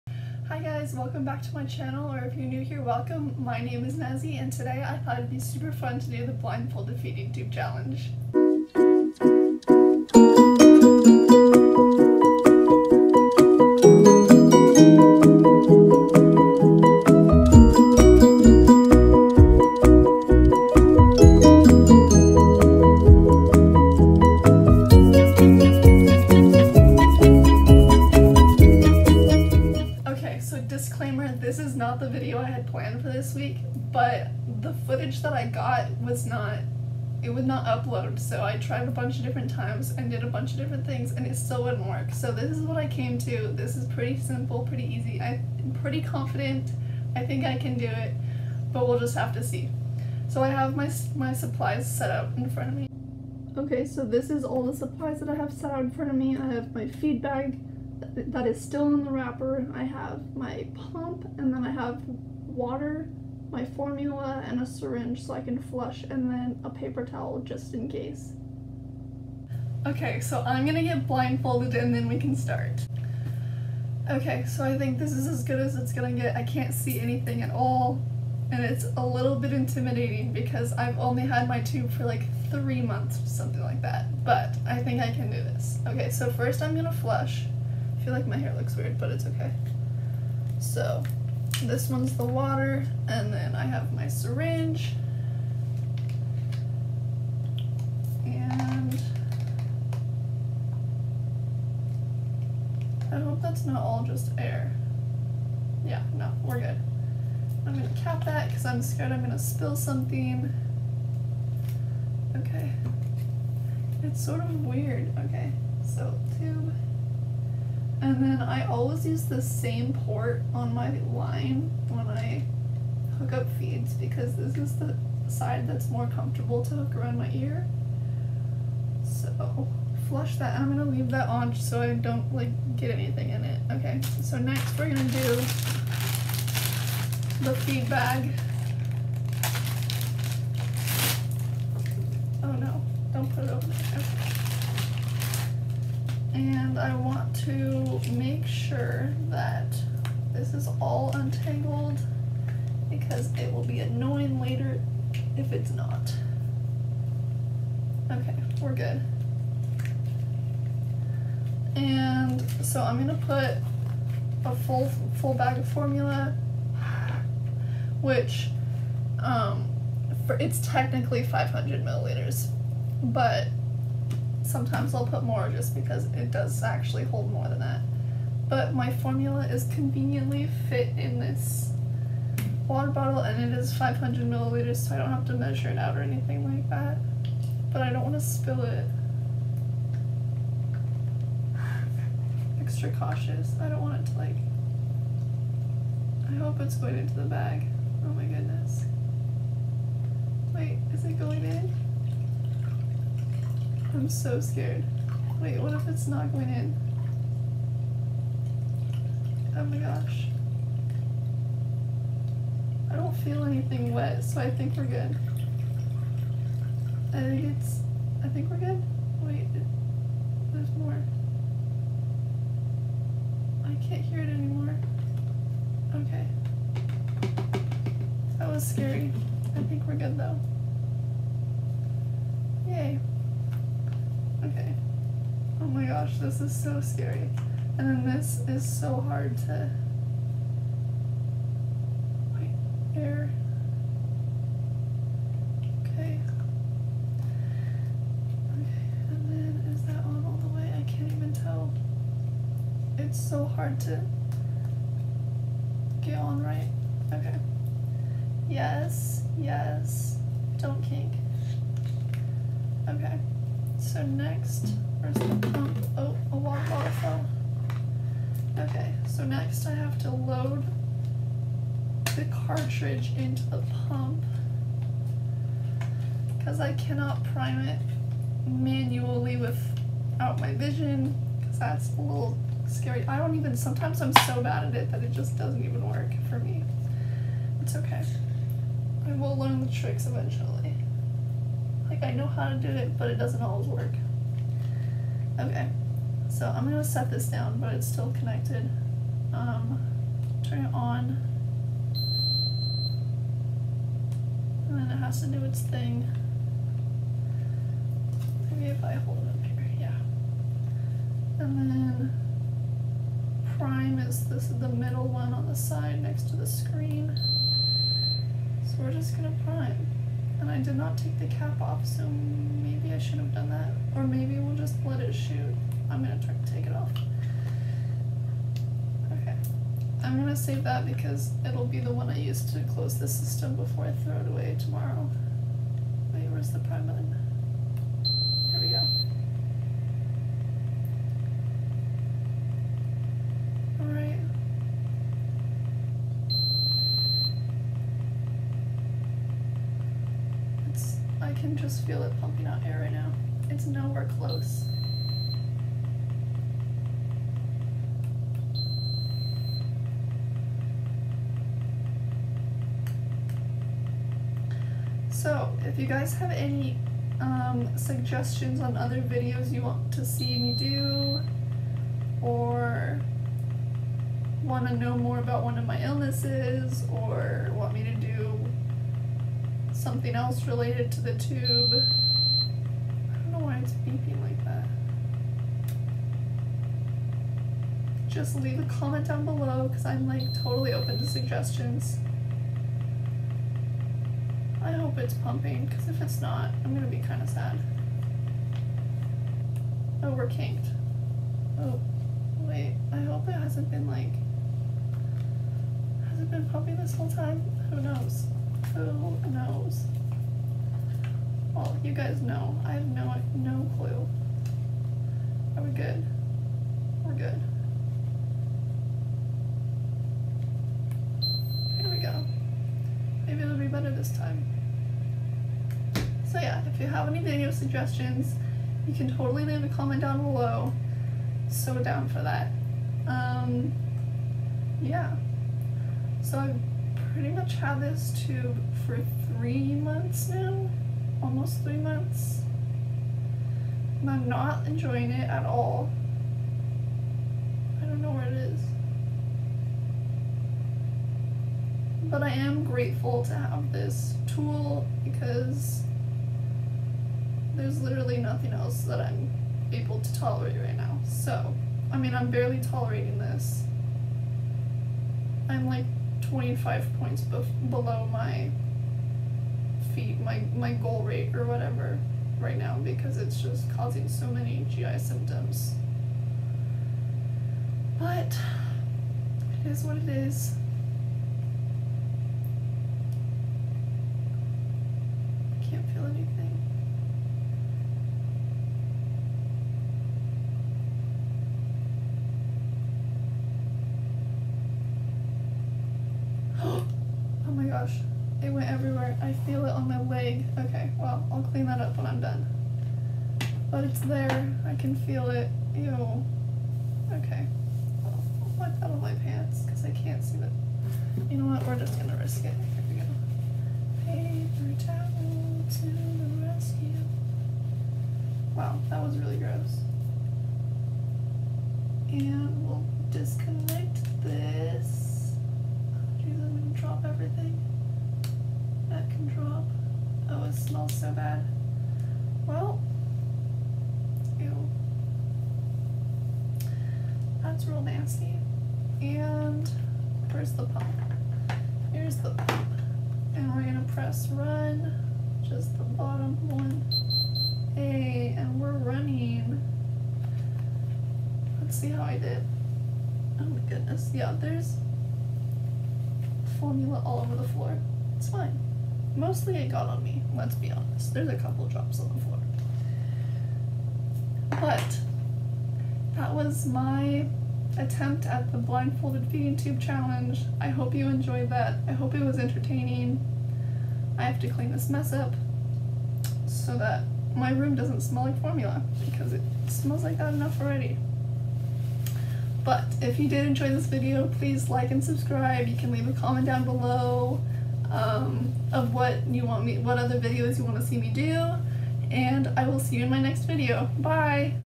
Blindfolded Feeding Tube Challenge
I’ve see a couple tubies do this challenge and thought it would be fun.  I hope it’s entertaining!